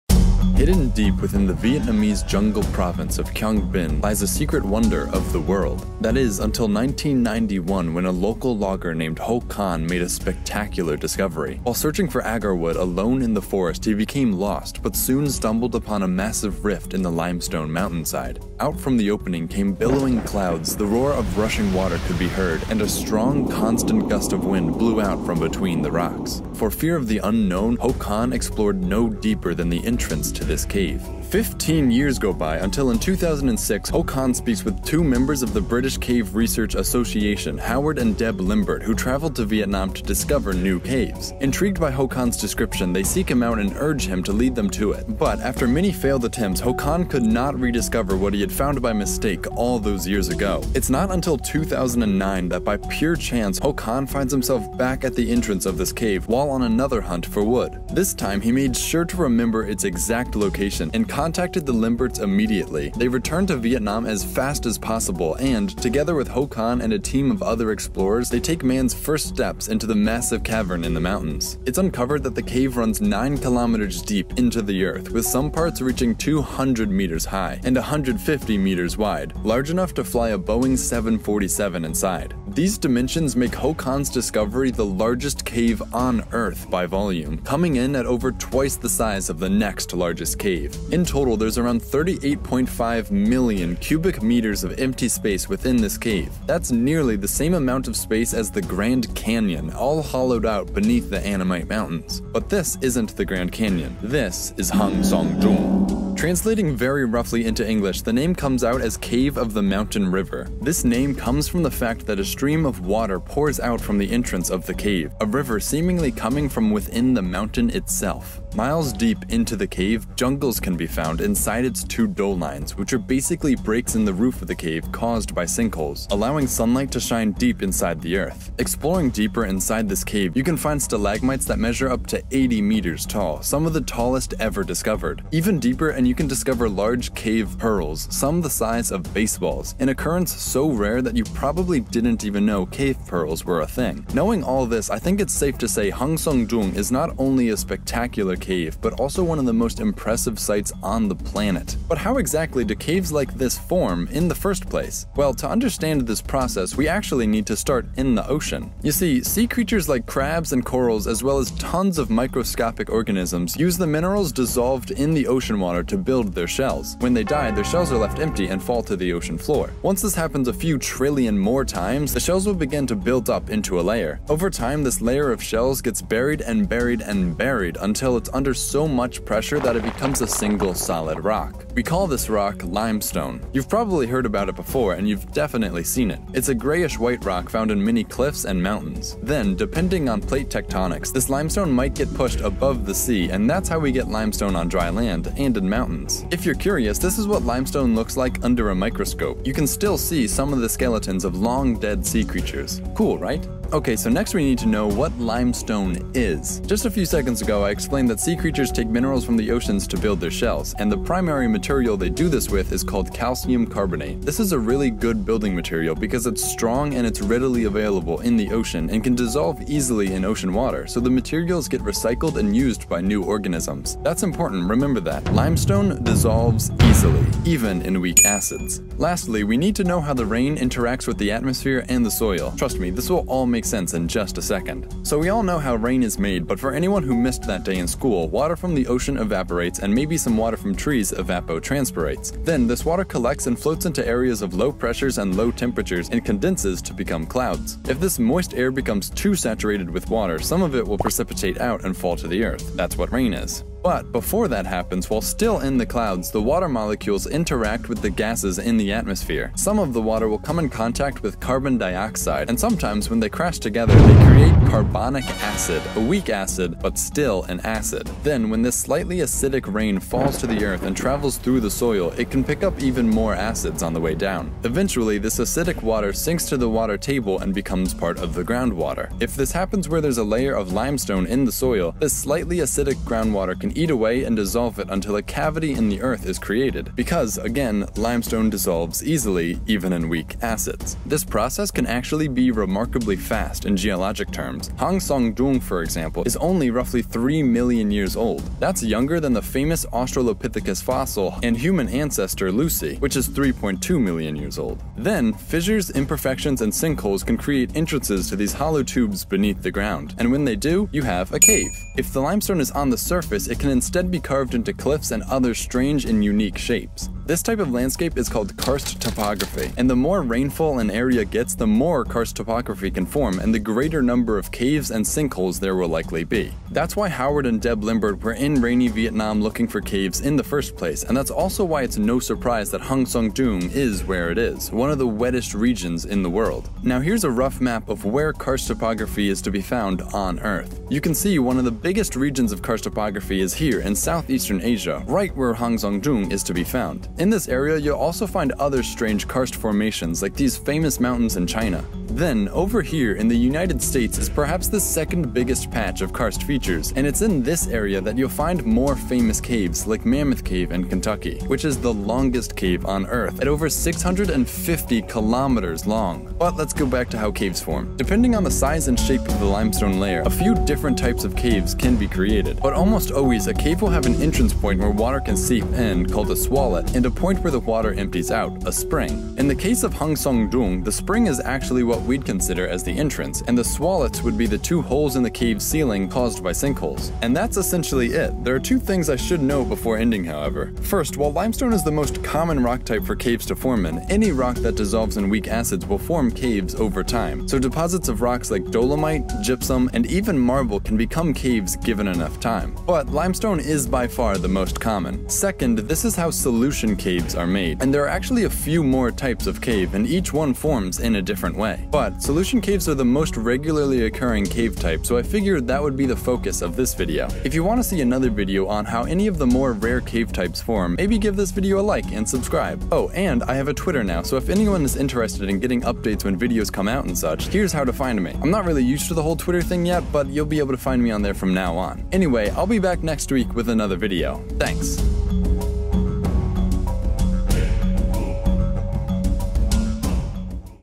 How Do Caves Form?
Caves are some of the most interesting and perplexing features on our planet. Here's how they form!

Oh yeah and I did the math wrong in comparing the size of the cave to the grand canyon, turns out I'm better at geology than I am at math, go figure. The grand canyon is actually MUCH larger.